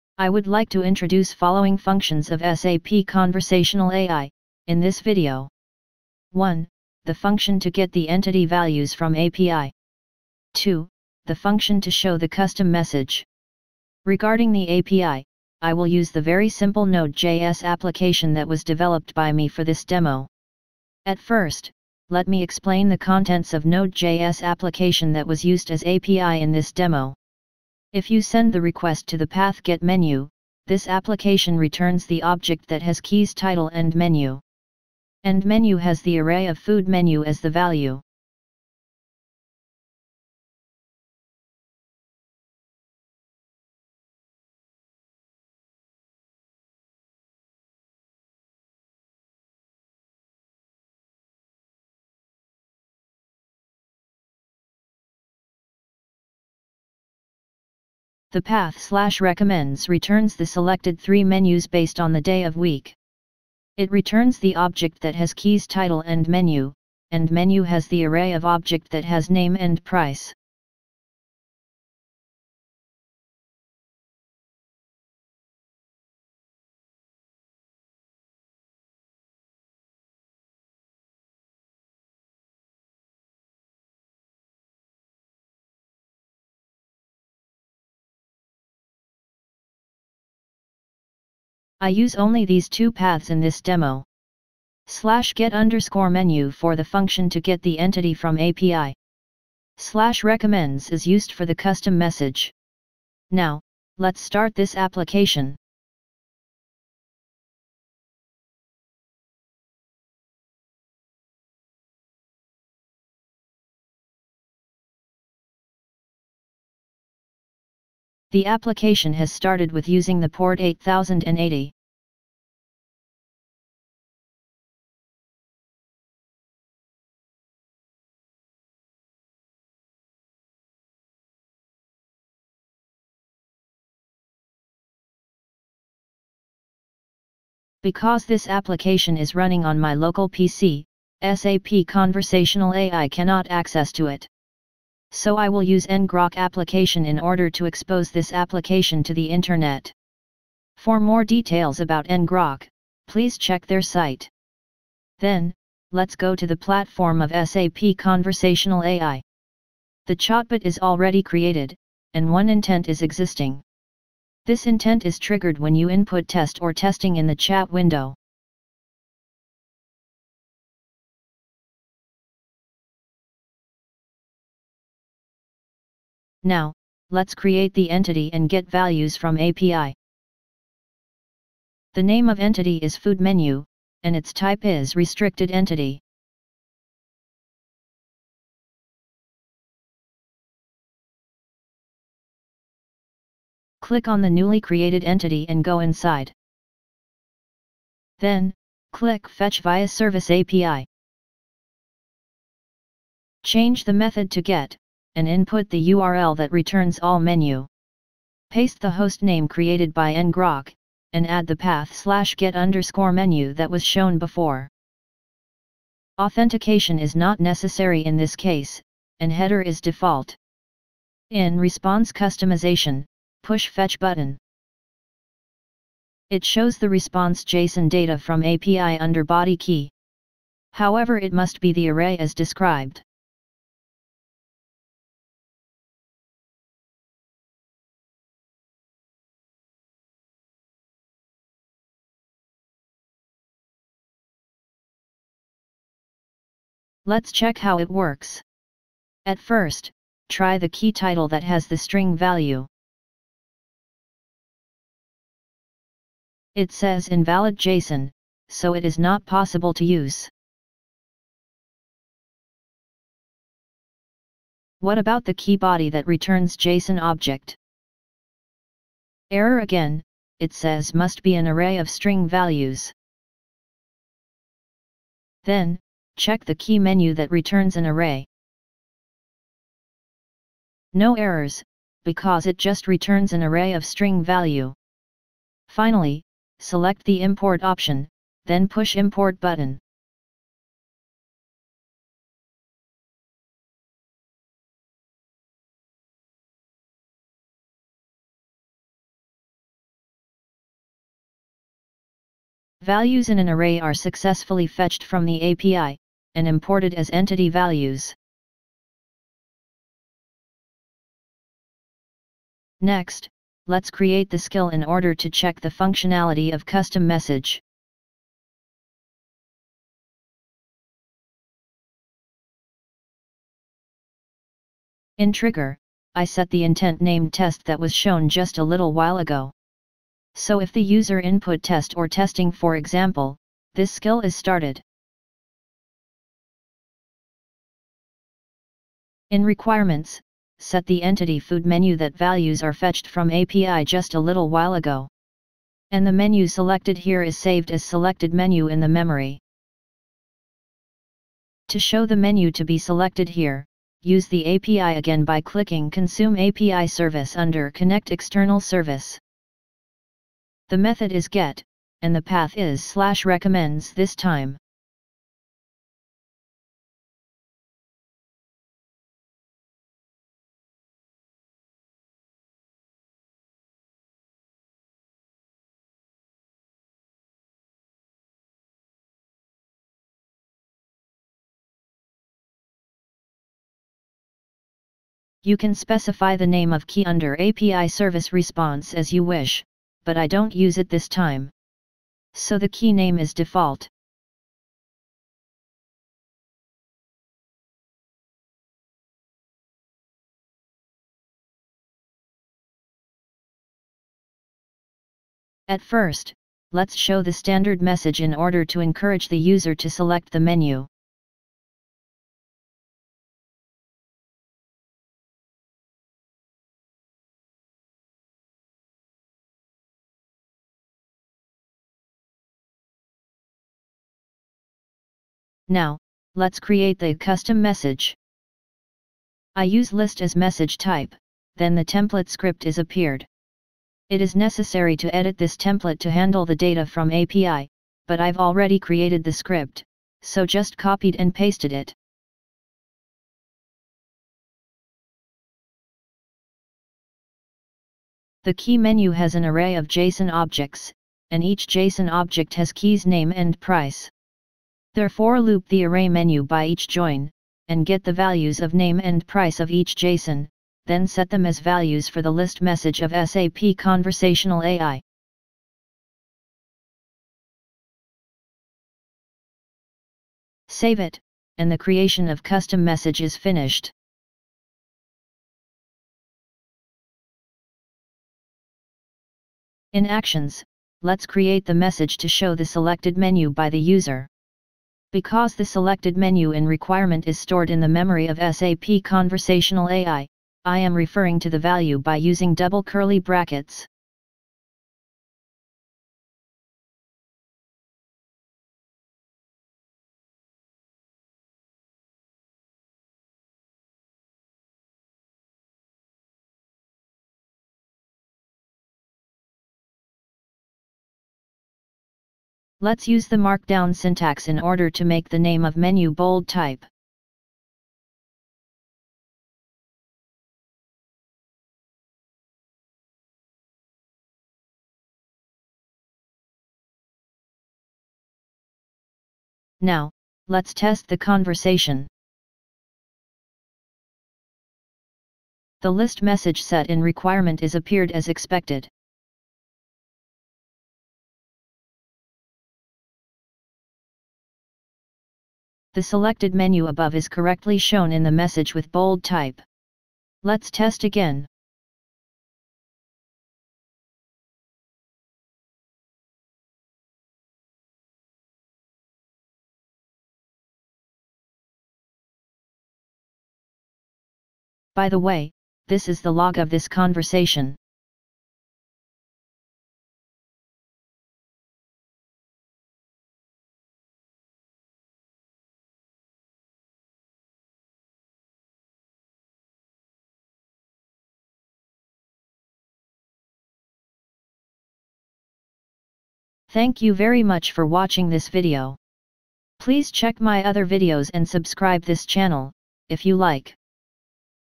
SAP CAI: Fetching Entity Values From API & Utilizing Custom Message Type
This video shows you how to use following 2 functions of SAP Conversational AI (CAI).
(1) Fetching Entity Values from API
(2) Utilizing Custom Message Type that handles data from API.

API used in this video is very simple self-developed Node.js Application (Javascript coding and uses Express Framework).

00:00 Prologue
00:21 Explanation of API (Node.js Application)
02:17 "ngrok" Command Operation
02:40 Intent Explanation of Demo Chatbot
02:56 Creation of Entity and Getting Values From API
05:12 Creation of Skill and Utilize Custom Message Type
07:59 Creation of the message in Actions
08:49 Testing of Chatbot Conversation